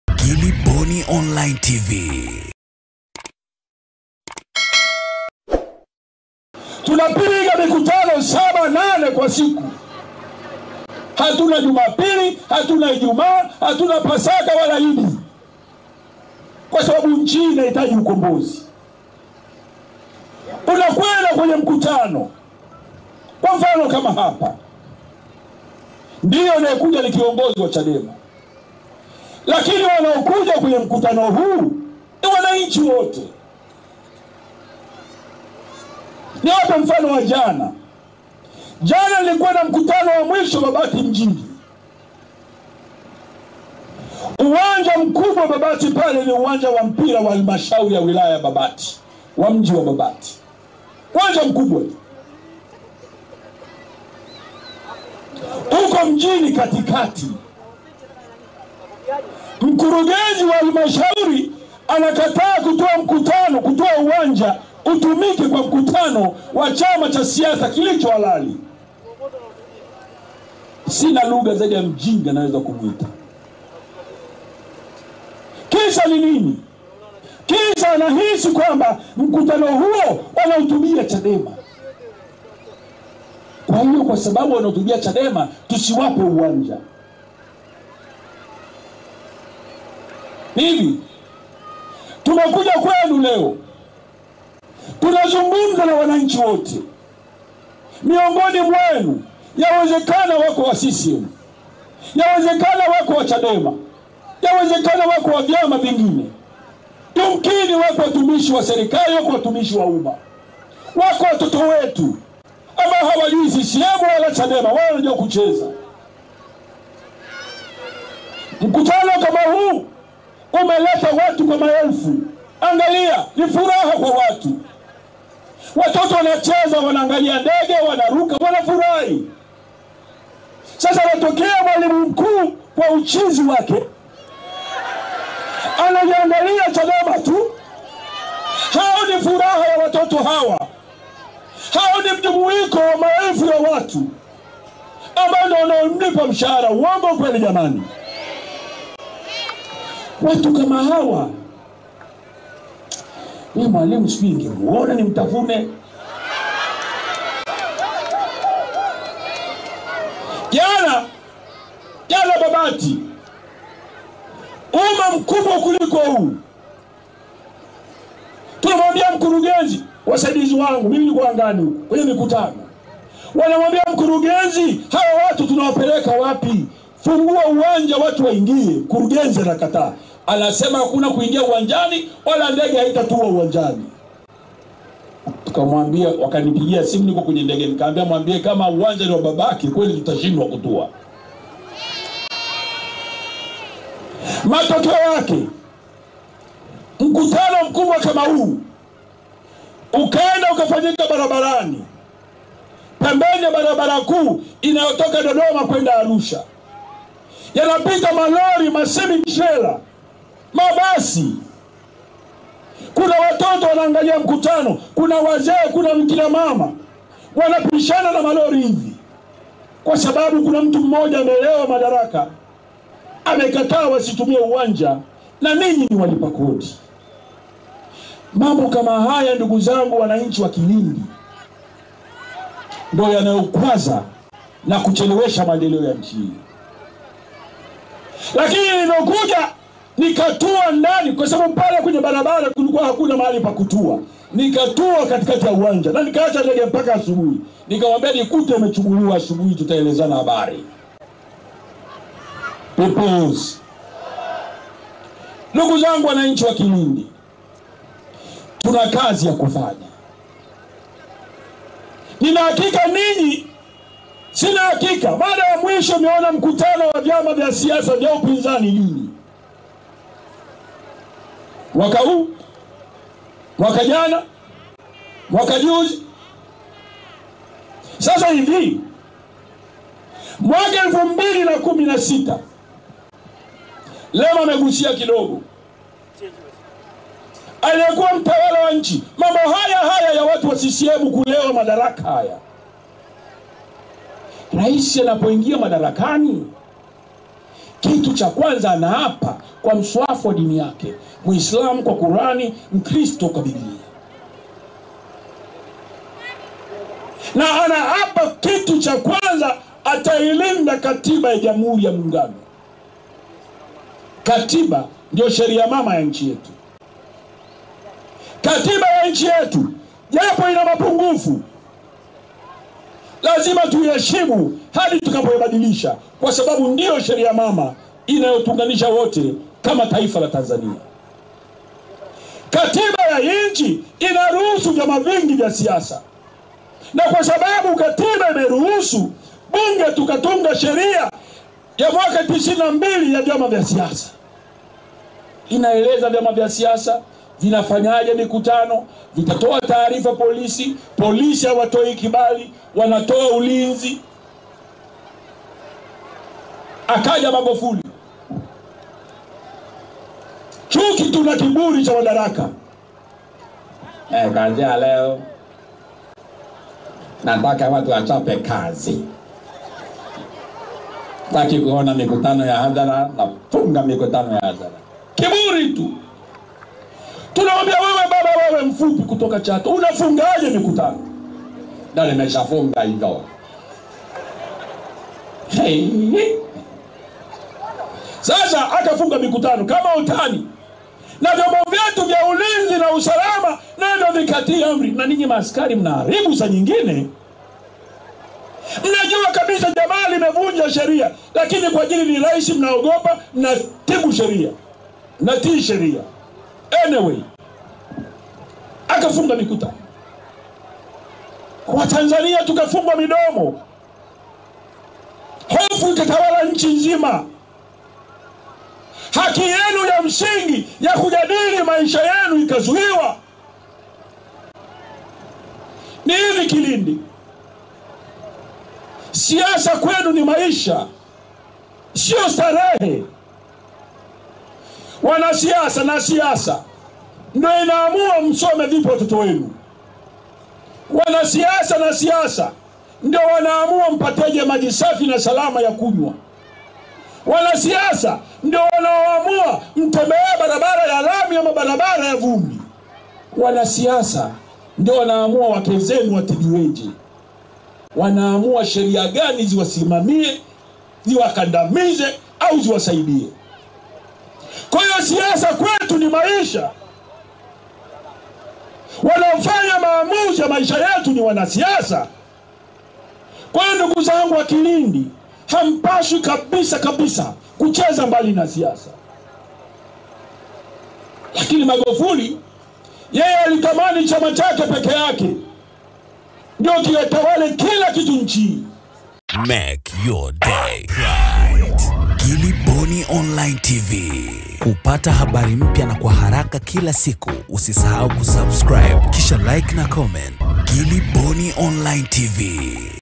Balaa la MBOWE Kwenye mkutano wa hadhara CHADEMA
MBOWE AKINUKISHA TANGA, AMDAKA MWALIMU MKUU ALIYEJARIBU KUZUIA MKUTANO WAKE, AMPA ZA USO